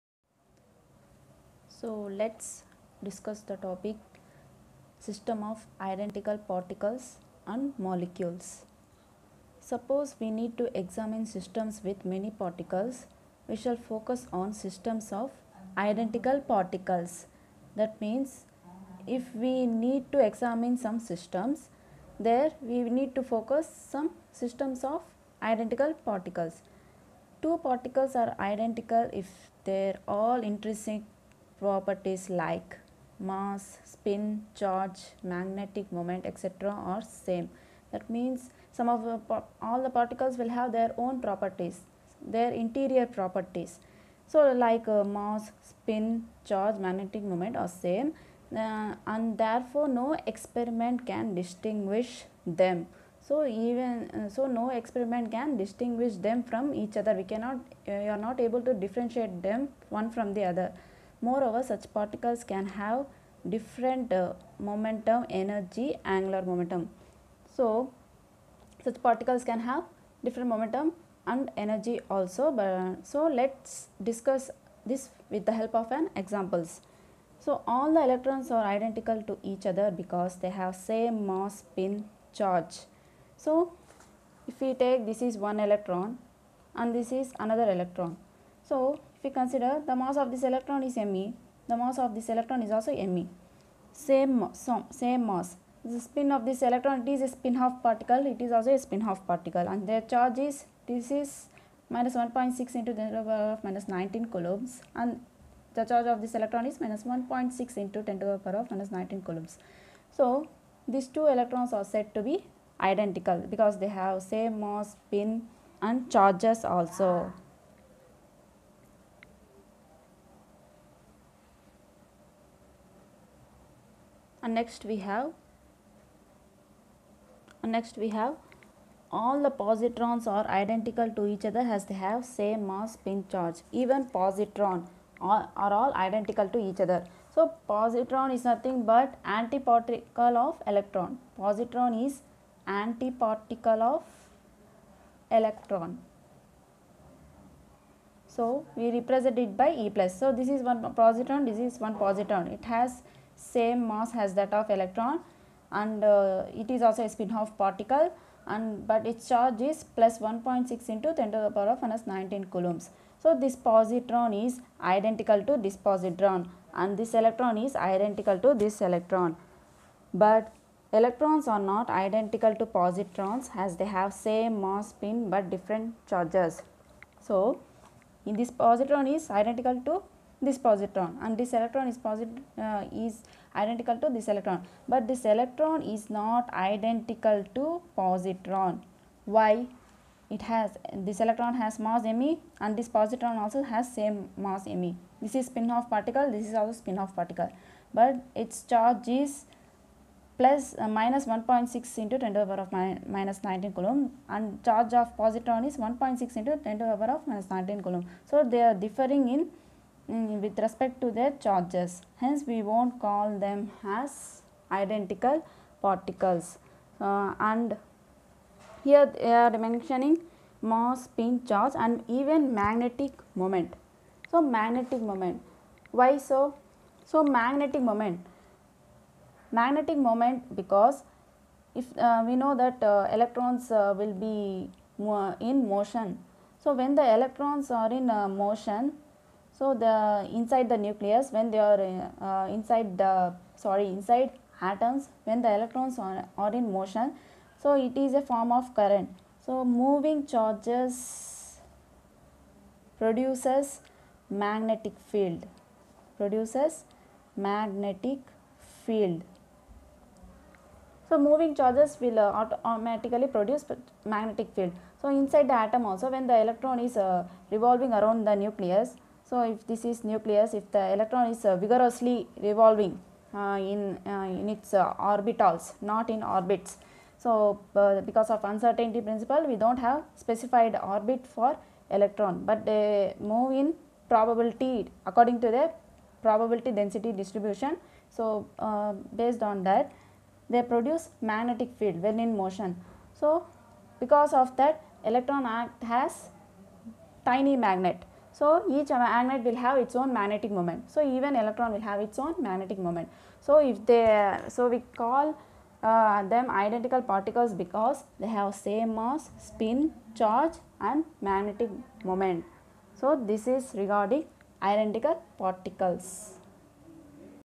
System of identical particles| NNN |QUANTUM MECHANICS
It's a discussion on identical particles. By this we come to know that all electrons are identical to each other. But electrons are not identical to positrons.